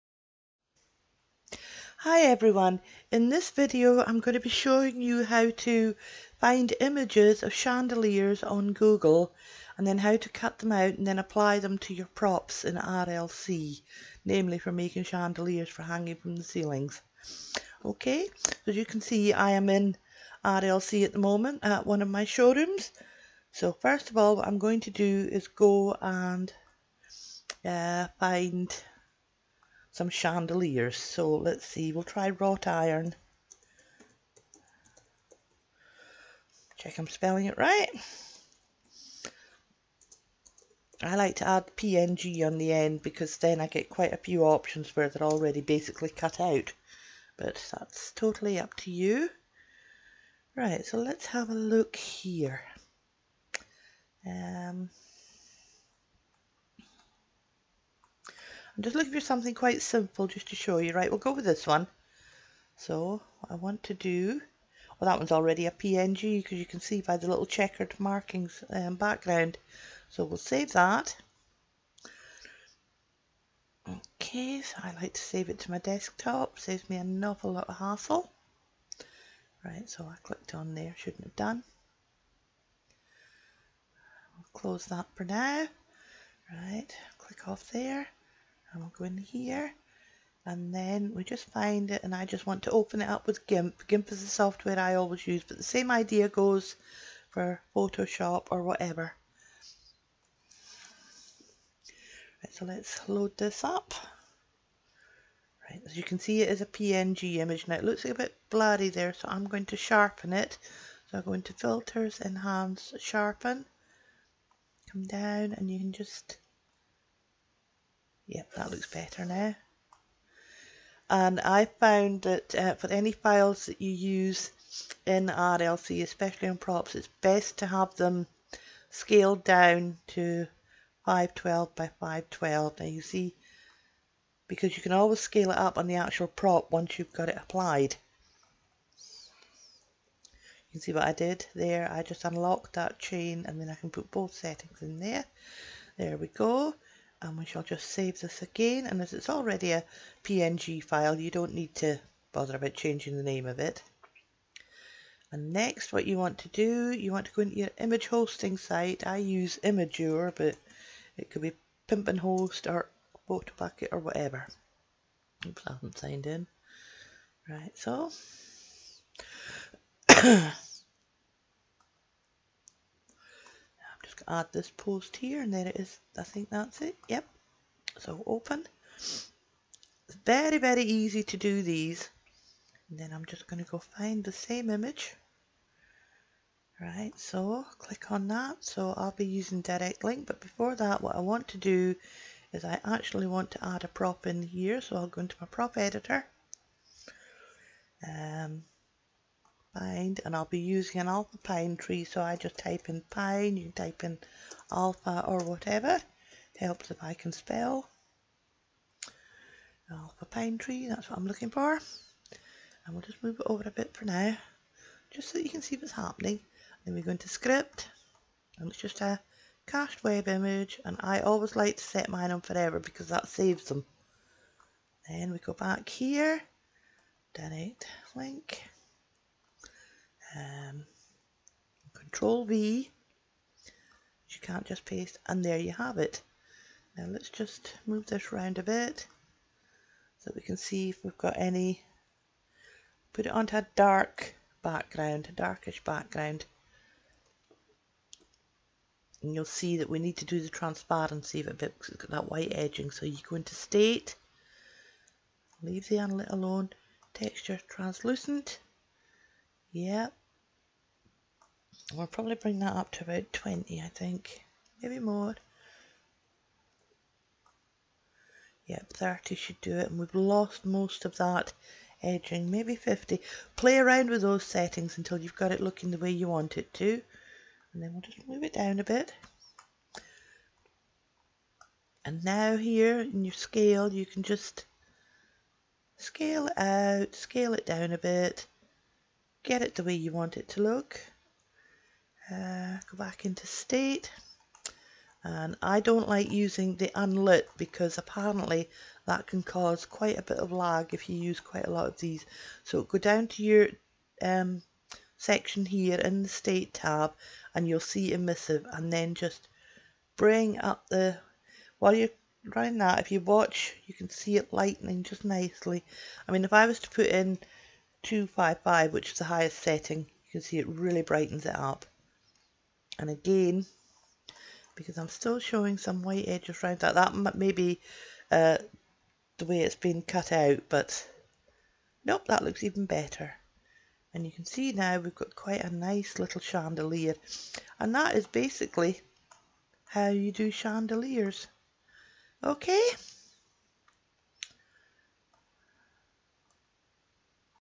CREATING A CHANDELIER PROP IN RLC
HOW TO MAKE A SIMPLE CHANDELIER PROP FOR USE IN RLC